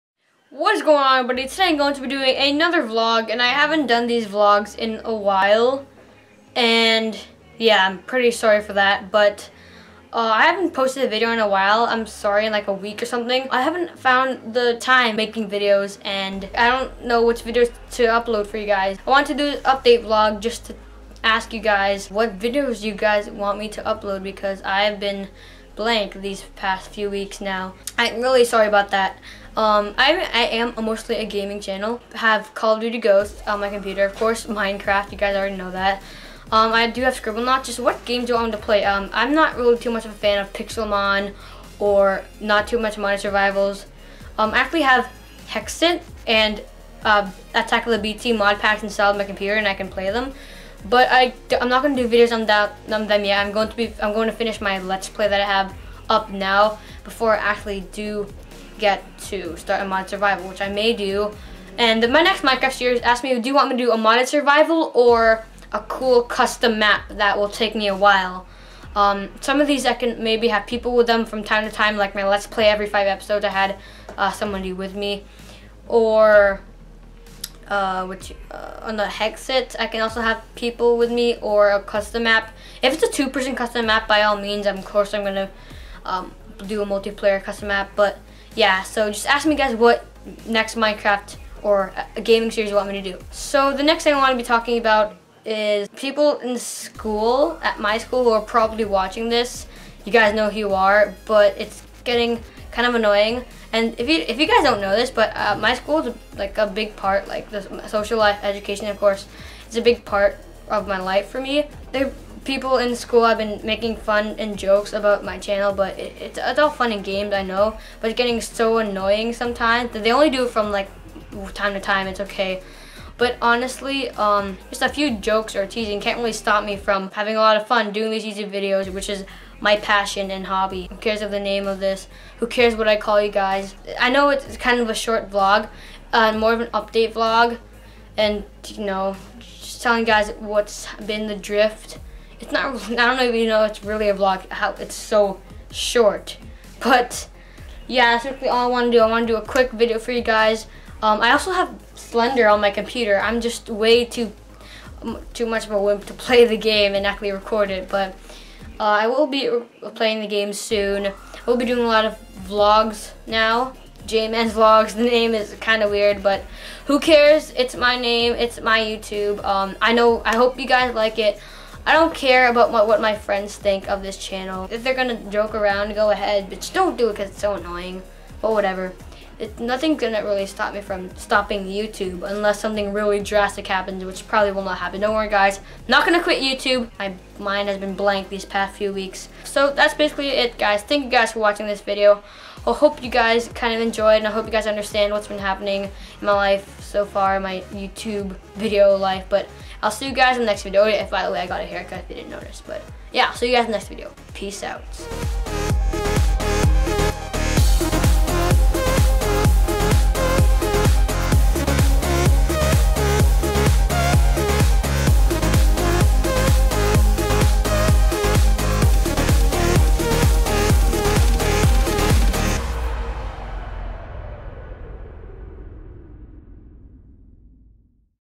Jman's Vlog! Week 12- Annoying Friends, Updates!
Hey Oreos, welcome to a different vlog, where I update you guys on what has been going on, but also I give a message I wanted to say for a while now, It's kind of a weight lifted off my shoulders I guess. But anyways, I thank all of you guys for being amazing subscribers, and amazing people.

Skype: justinantony11
Minecraft Name: justinorial
Game Center: JustinBoy11